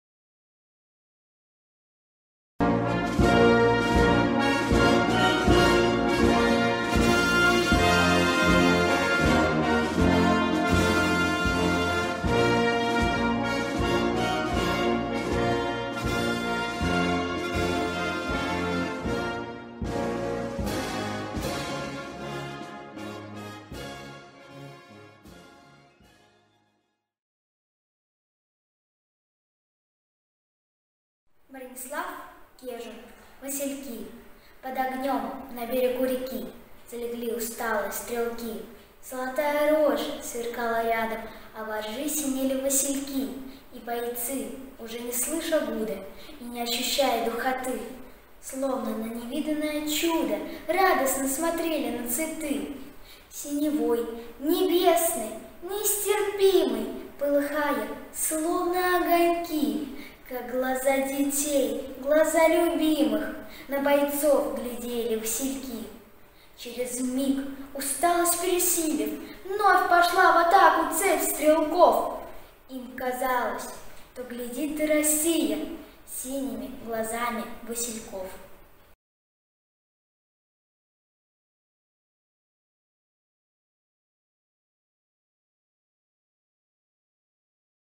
Бронислав Кежун «Васильки». Читает Александра Филиппова, 9 лет, г. Остров Псковской области.
Бронислав Кежун «Васильки».
Читает Александра Филиппова, 9 лет, г. Остров Псковской области.
Руководитель учитель начальных классов Кабатчикова Татьяна Николаевна

Бронислав Адольфович Кежун (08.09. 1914, Петроград — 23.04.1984, Ленинград) — советский поэт и переводчик, журналист, военный корреспондент. Член Союза писателей СССР с 1939 года. В годы Великой Отечественной войны служил военным корреспондентом в газетах Карельского, Третьего Украинского и Второго Дальневосточного фронтов. Войну закончил на Дальнем Востоке.
 Награждён Орденом красной звезды и Медалью «За победу над Германией в Великой Отечественной войне 1941–1945 гг.» В сборниках поэта отражены его фронтовые пути. В Беломорске вышли его стихотворные сборники: «Рассказы северных гор» (1942), «Красноармейские стихи» (1943), «Рассказ о русском богатыре» (1943) года.